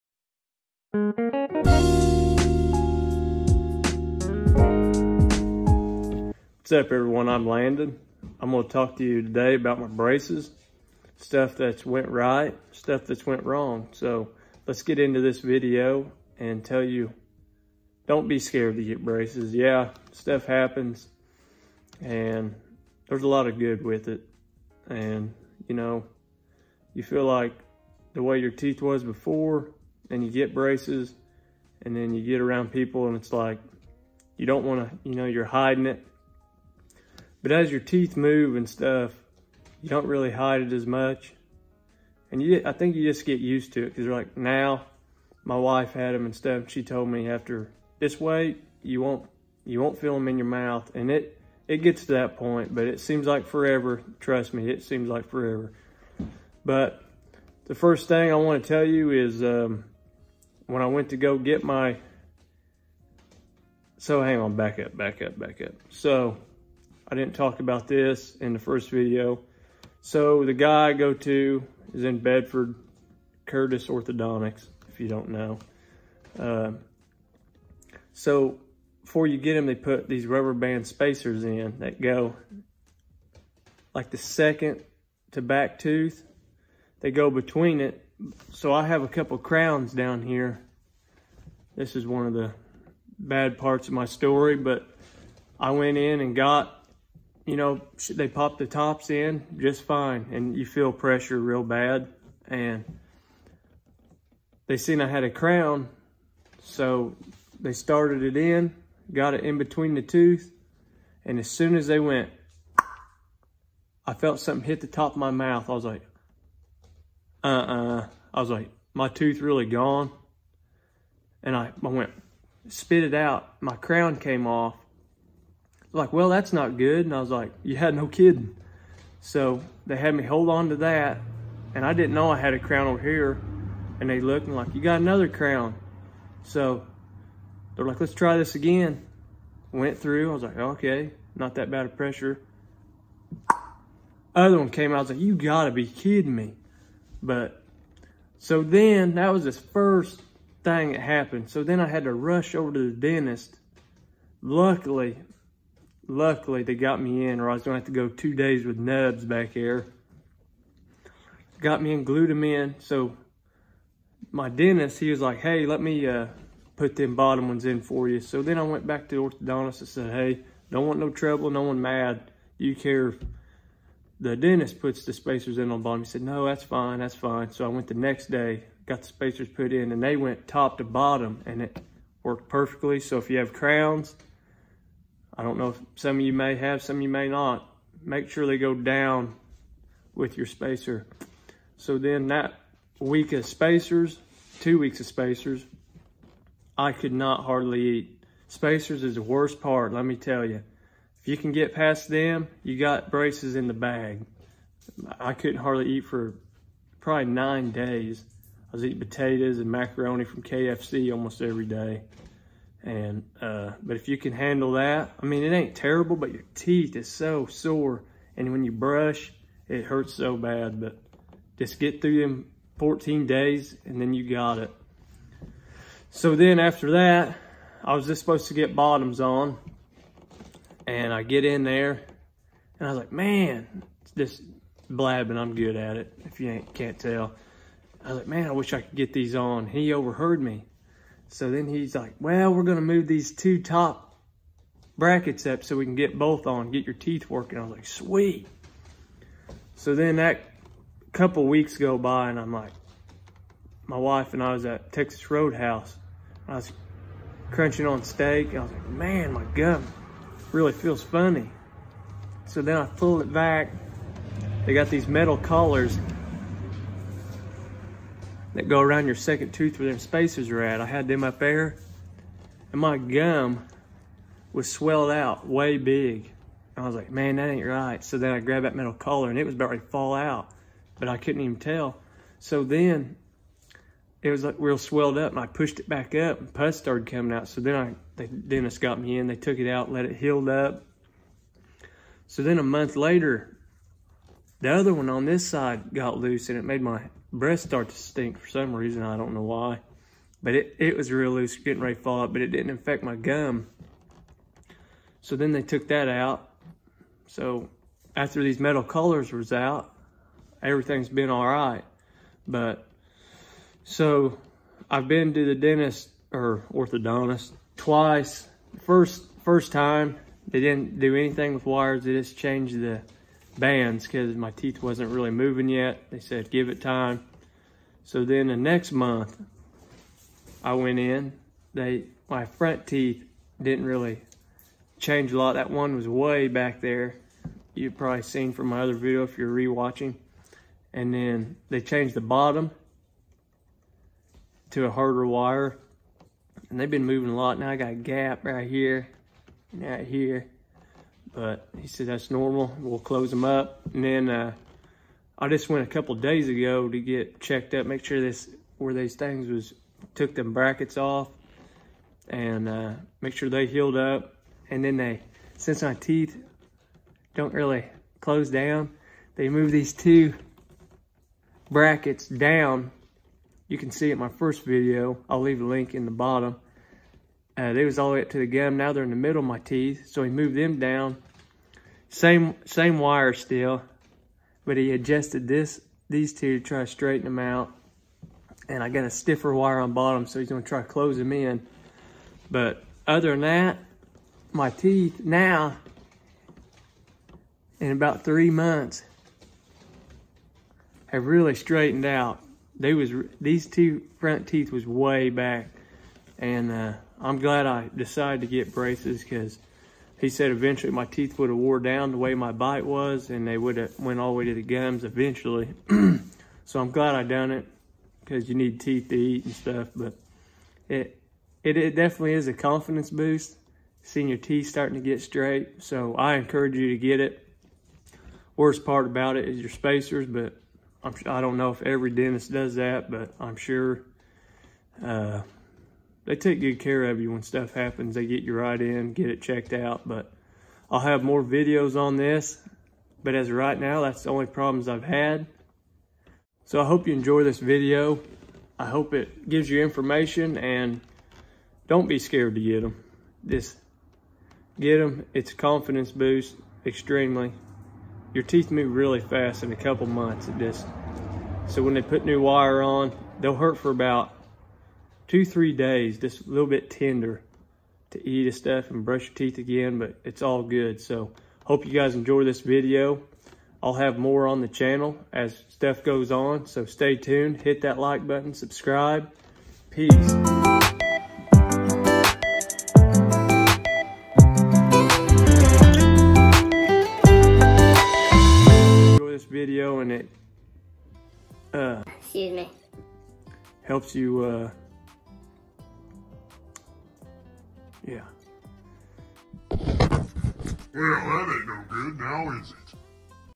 Braces-Explained, Complications And Changes In 3 Months
I’ve almost had them three months now and I’m very pleased with the results so far got Akon at to go